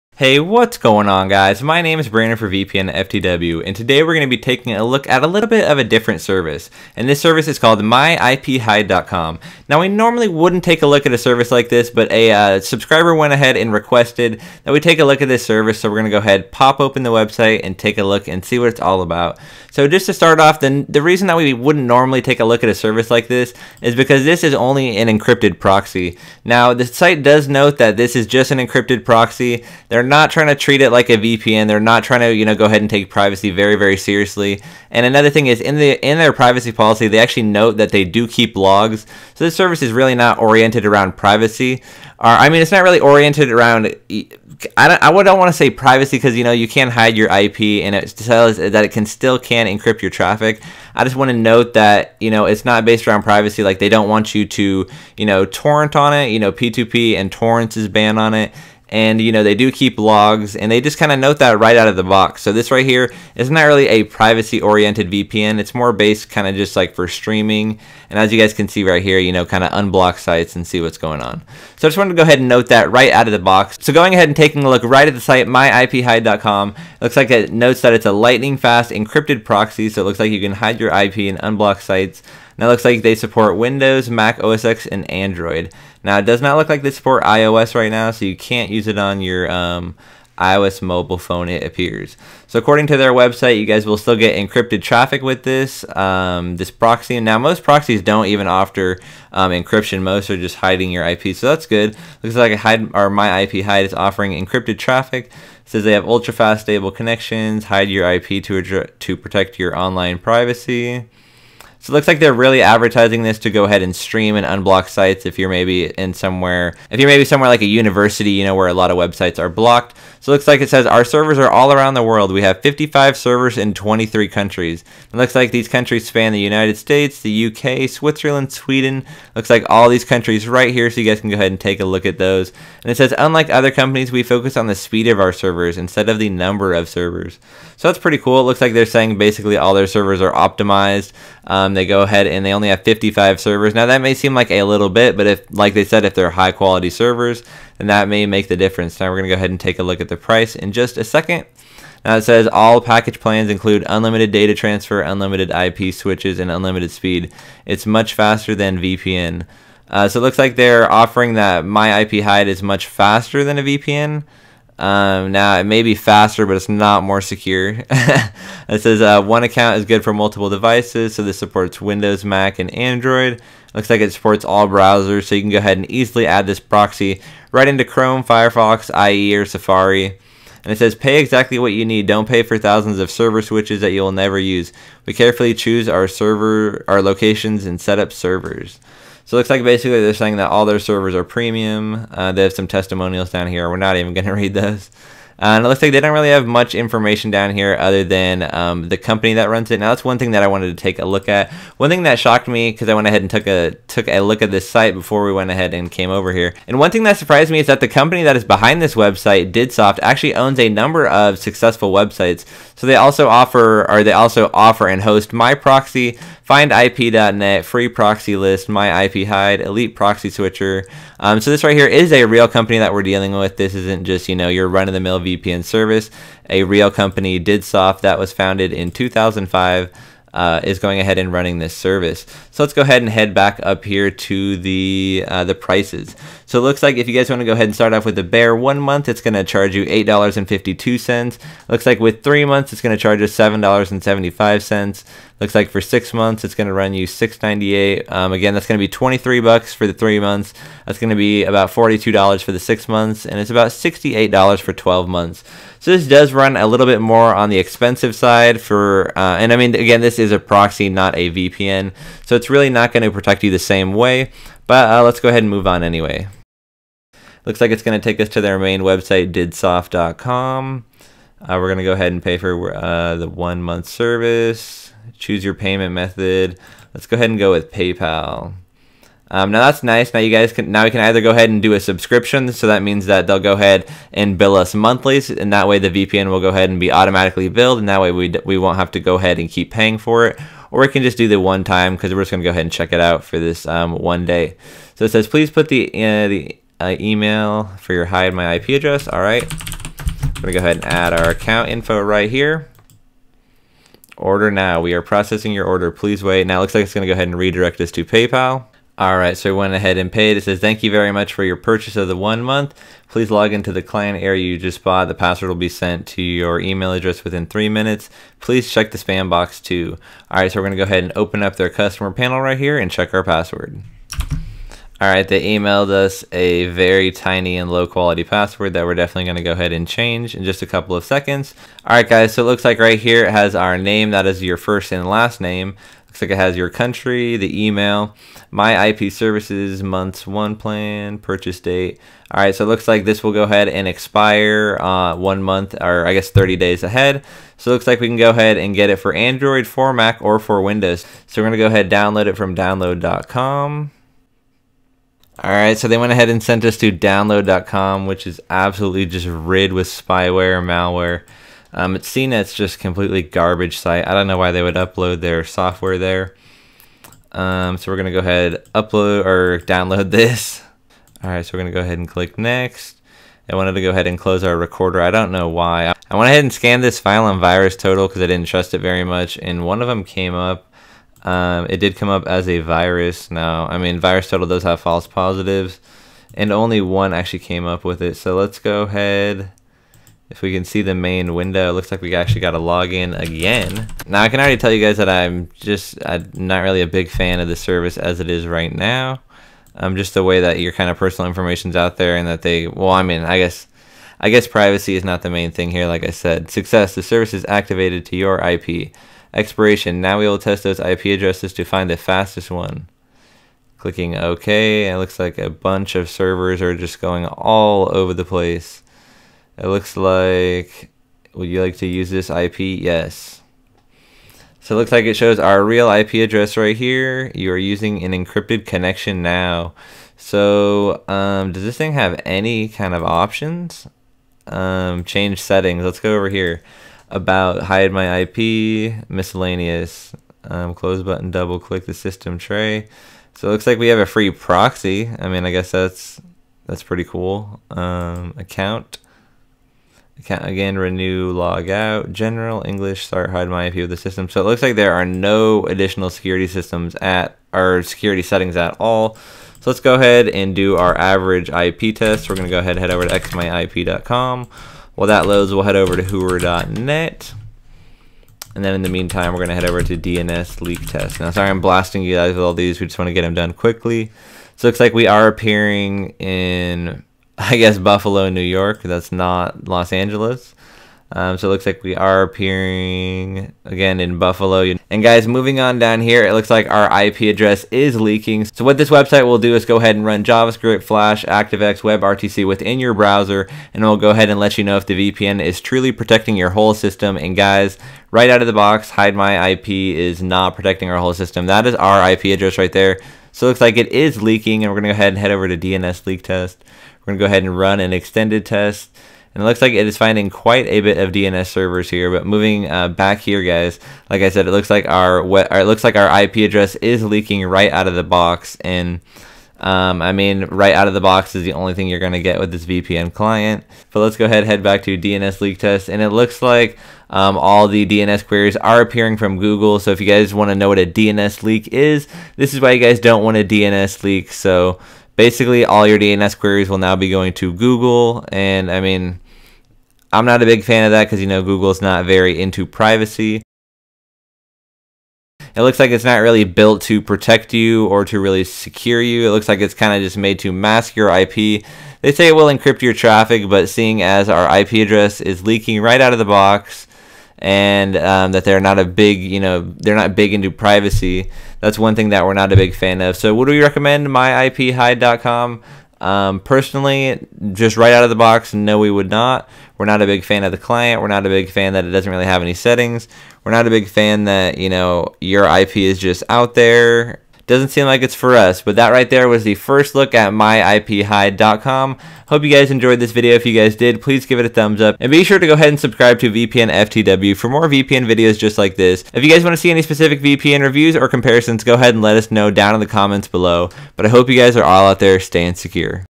First Look at MyIPHide in 2016
Taking a first look at MyIpHide, the encrypted proxy!